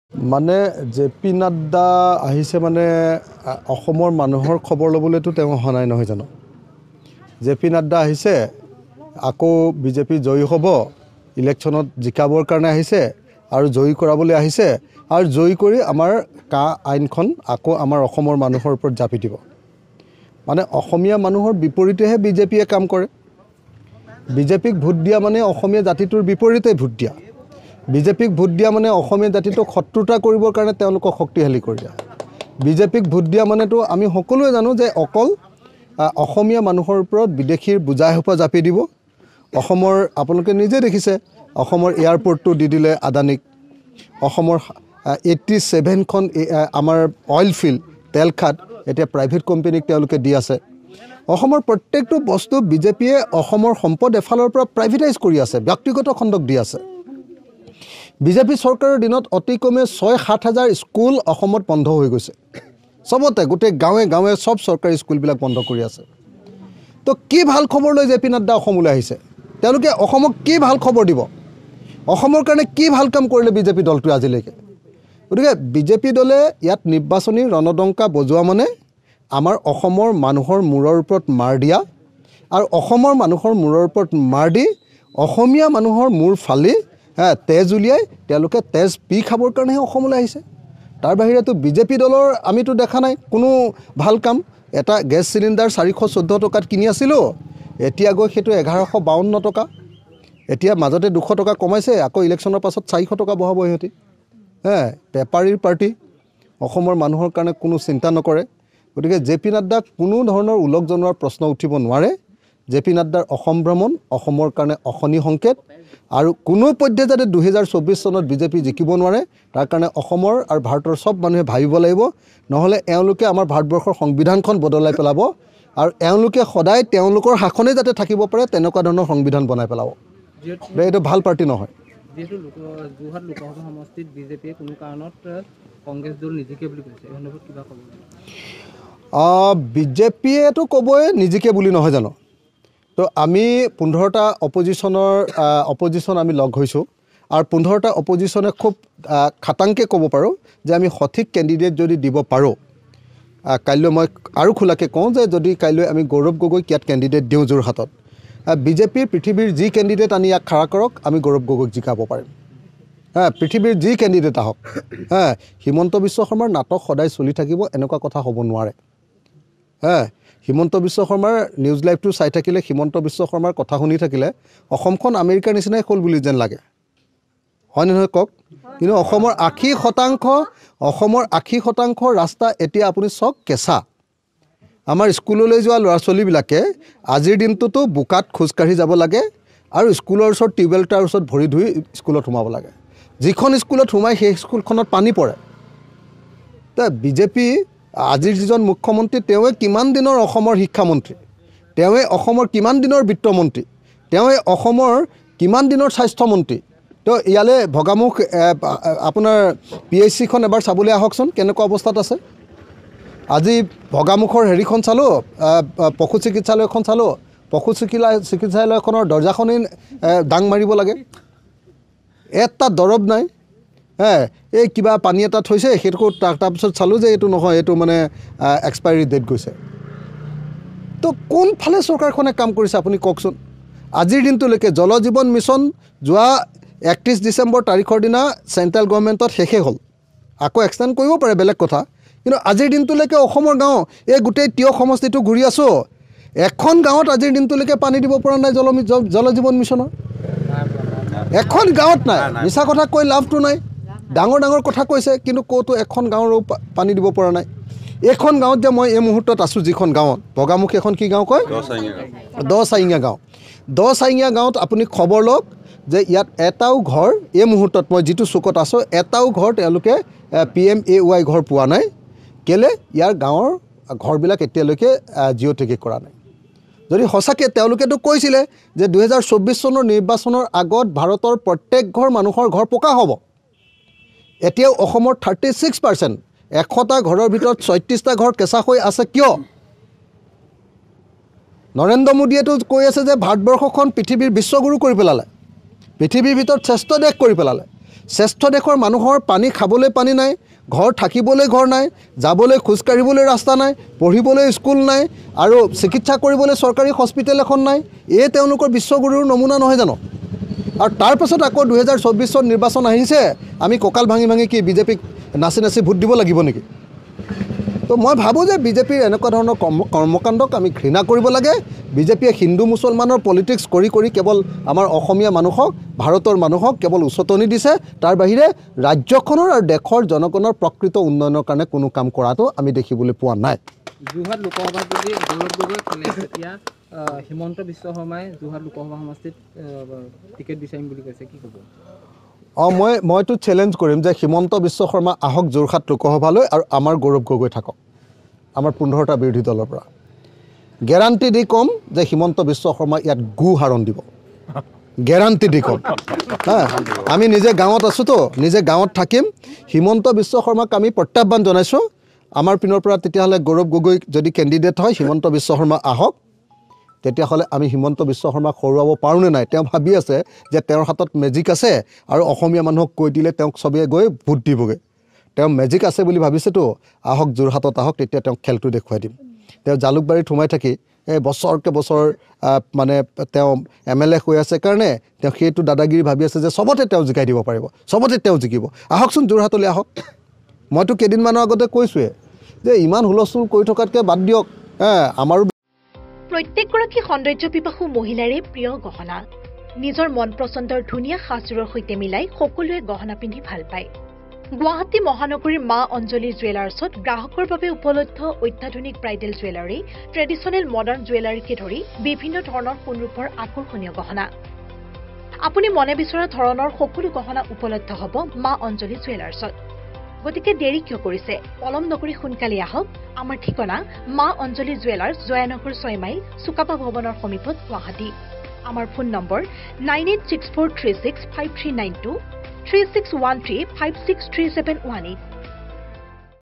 ‘’জে পি নাদ্দা অসমলৈ অহাটো অসমৰ মানুহৰ বাবে অশনি সংকেত’’
মুখ্যমন্ত্ৰীয়ে যোৰহাটৰ পৰা লোকসভা নিৰ্বাচন খেলক-অখিল গগৈ